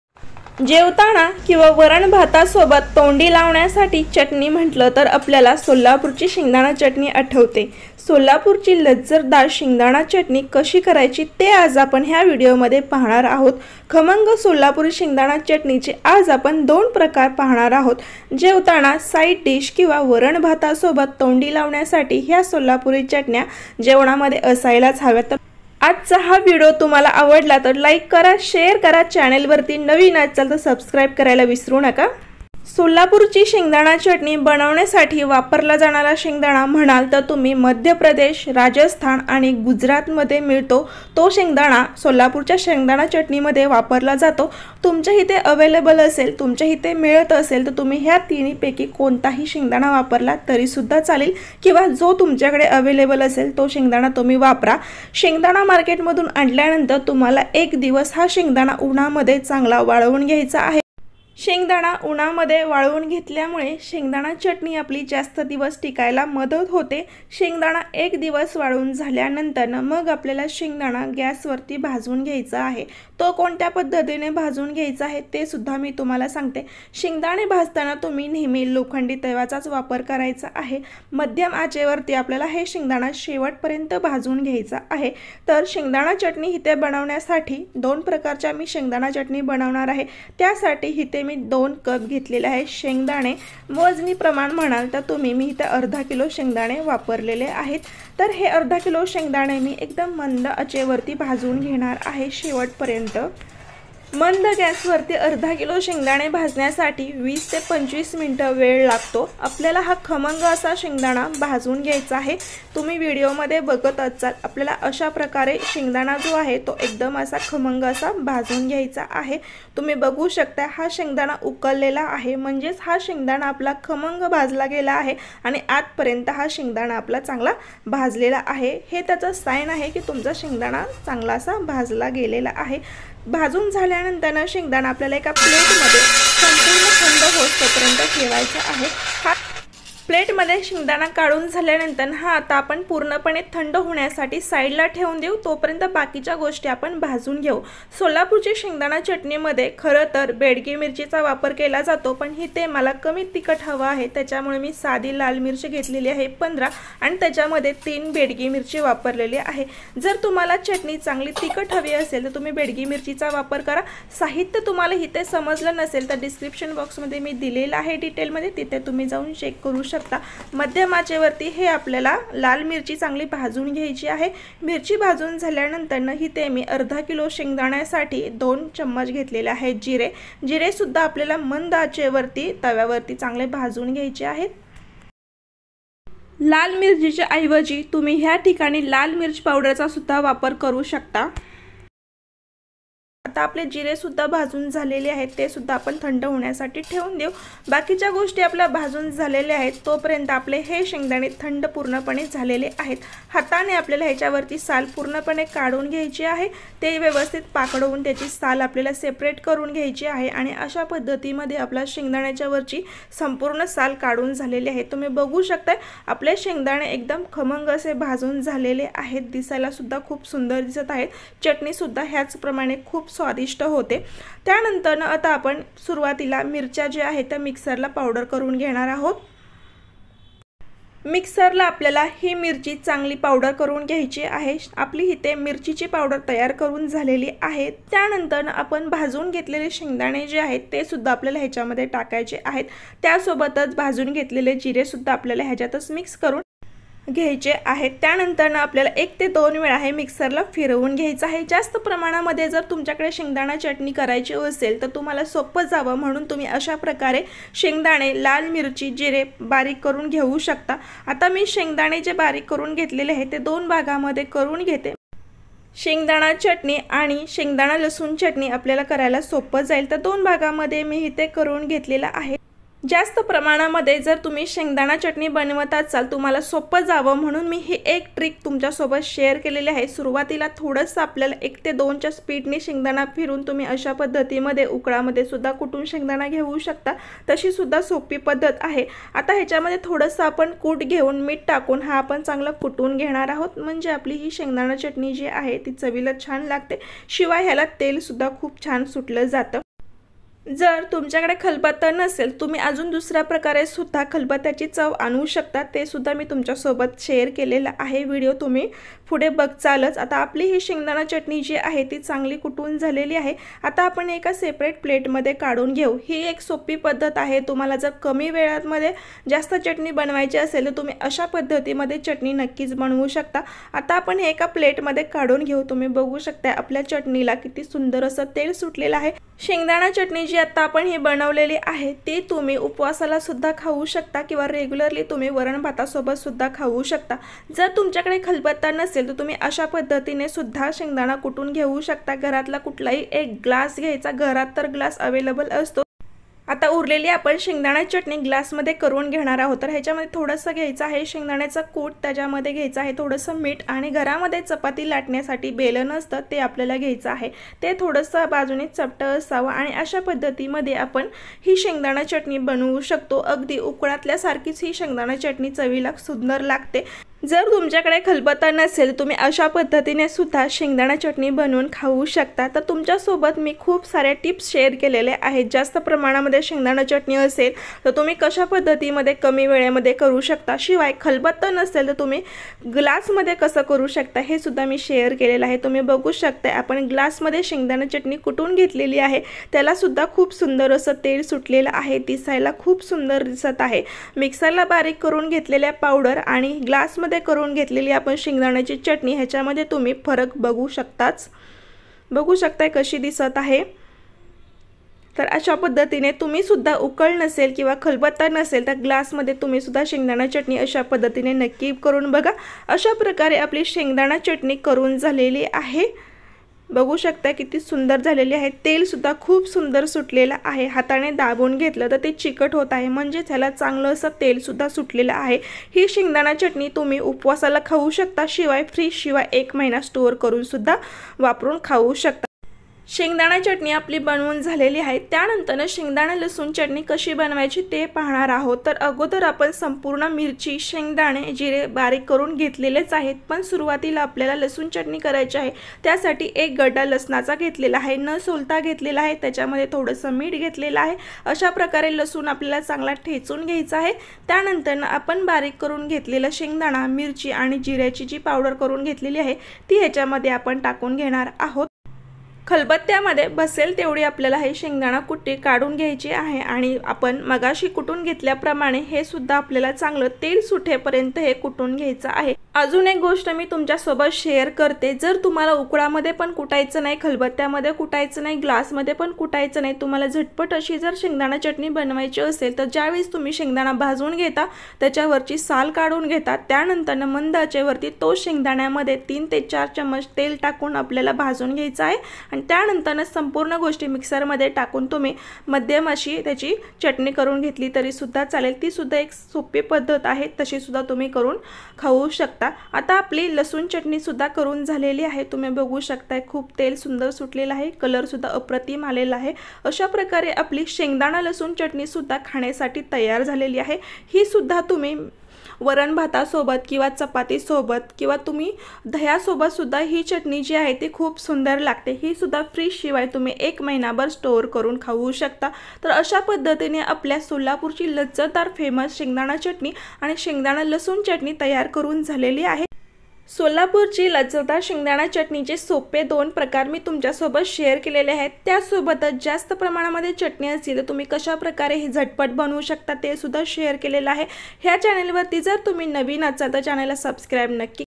खमंग सोलापुरी शेंगदाणा चटणीचे २ प्रकार खलबत्ता नसेल तर ह्या सोप्या पद्धतीने आणा खलबत्ता चटणीची चव.
शेंगदाणा चटणी बनवण्यासाठी लागणारे साहित्य

२ कप शेंगदाणे (५०० ग्रॅम)
चवीनुसार मीठ
२ चमच जीरे
२ गड्डी लसूण
१५ नग लाल मिरची+३ बेडगी लाल मिरची